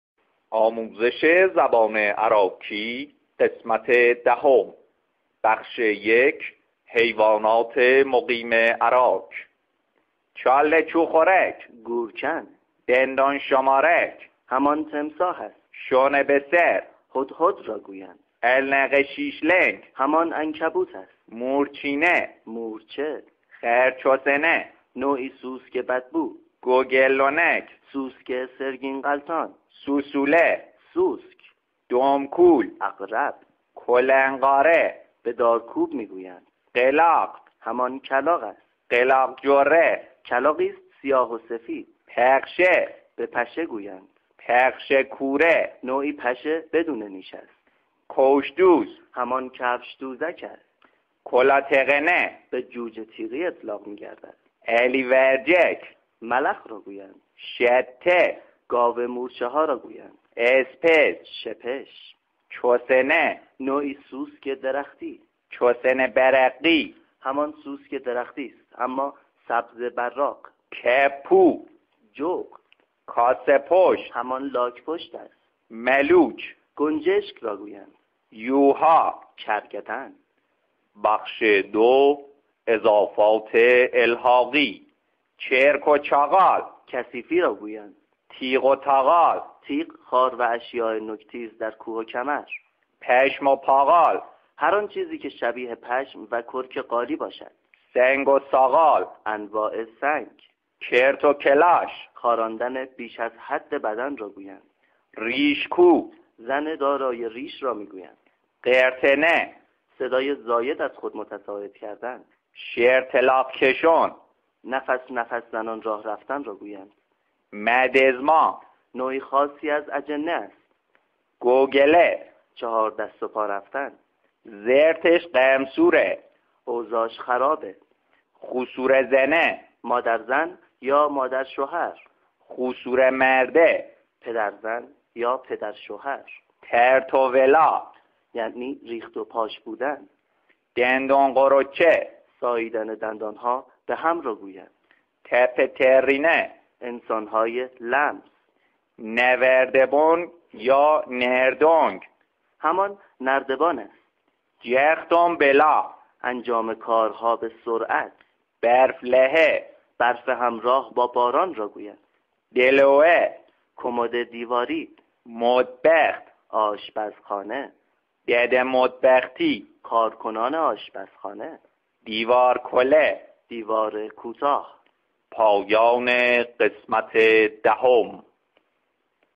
آموزش زبان اراکی ۱۰
آموزش زبان اراکی قسمت دهم - معرفی حیوانات مقیم اراک، معرفی اضافات الحاقی